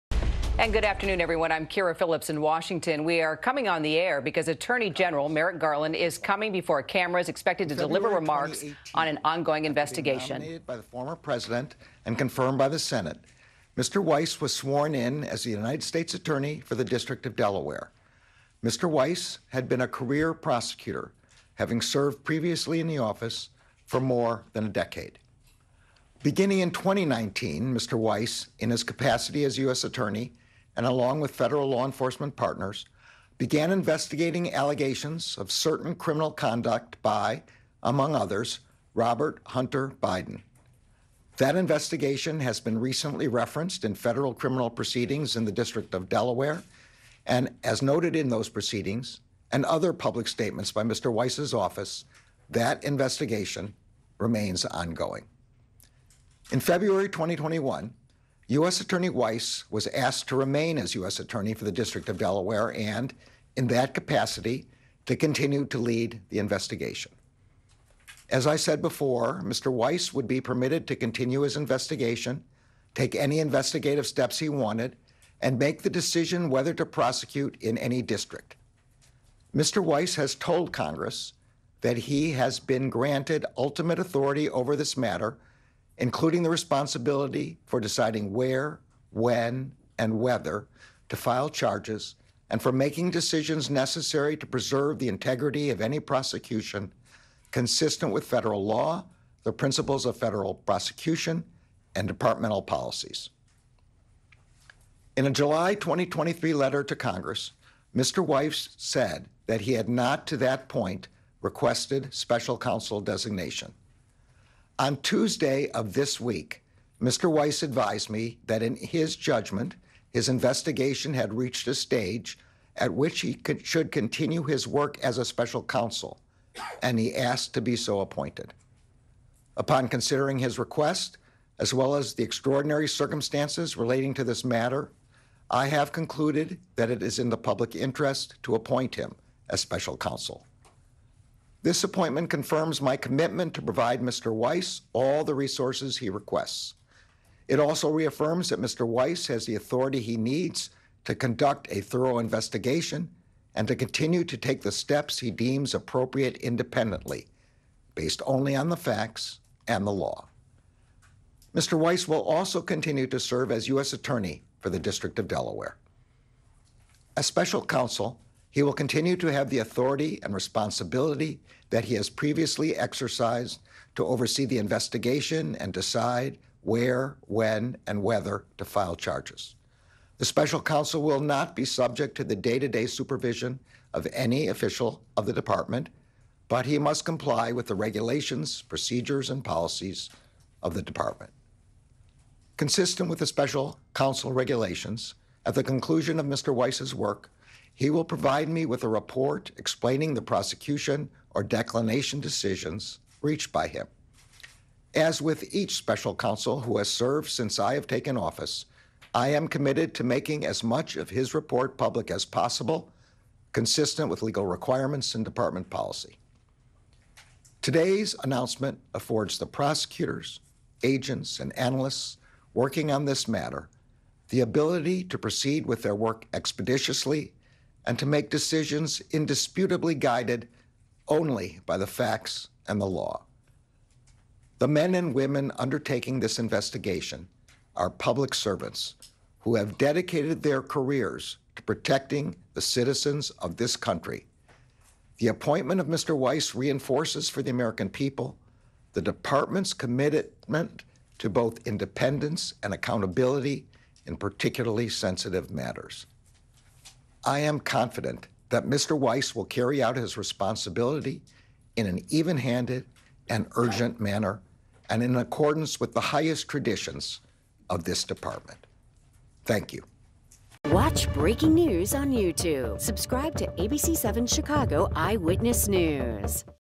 Hunter Biden prosecutor elevated to special counsel, US Attorney General Merrick Garland says
U.S. AG Merrick Garland appointed David Weiss as special counsel to oversee the Hunter Biden investigation.